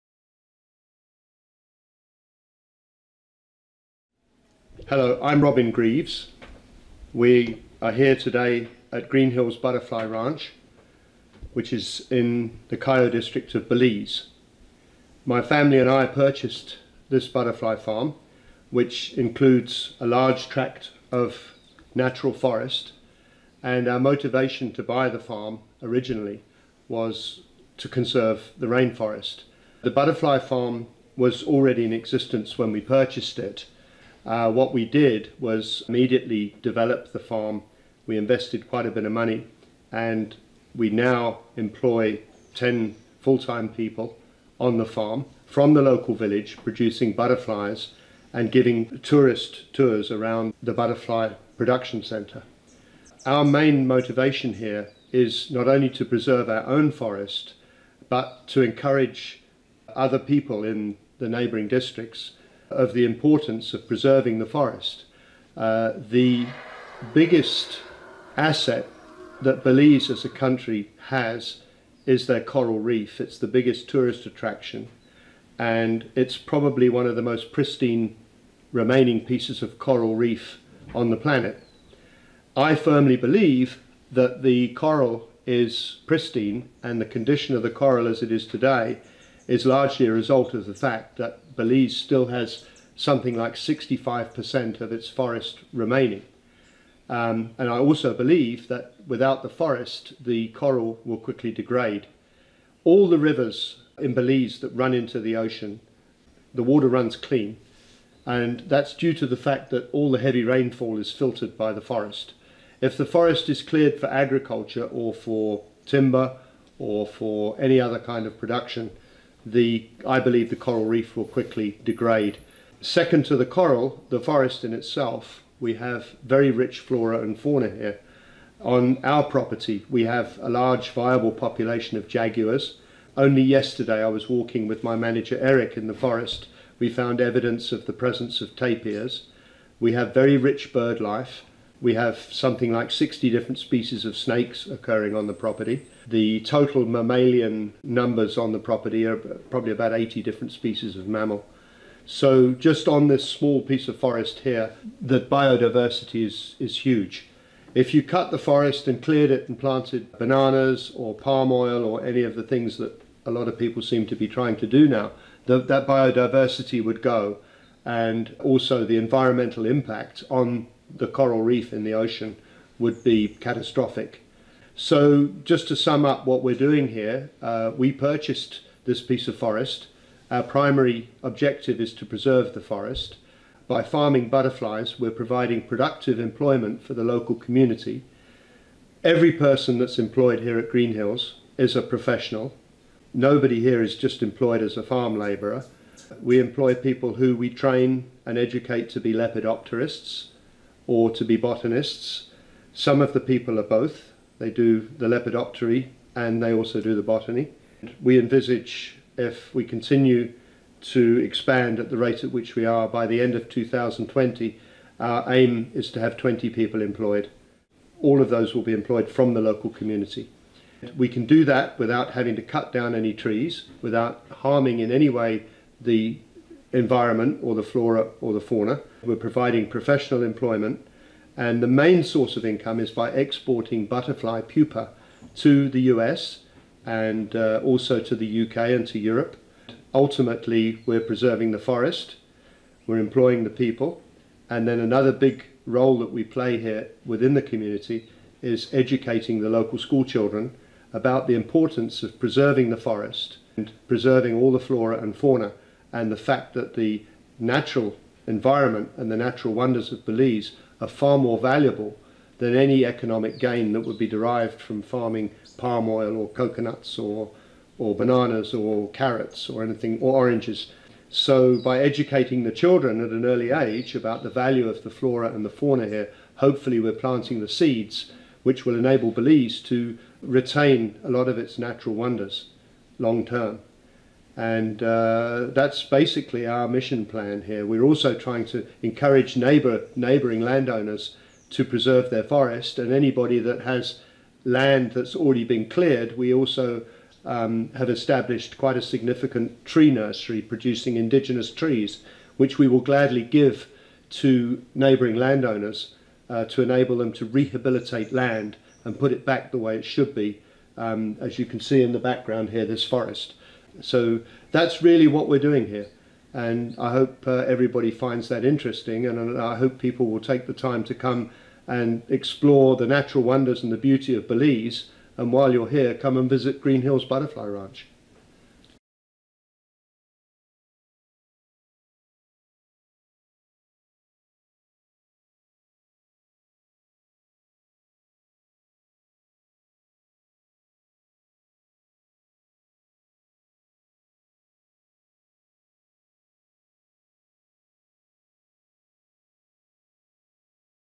GREEN HILLS BUTTERFLY RANCH MISSION STATEMENT
The Green Hills Butterfly Ranch is trying to make a difference, they all truly believe in Robins statement about forest survival and Belize's stake in it all. Great people doing a huge job, visit if you can .. share the video if you can not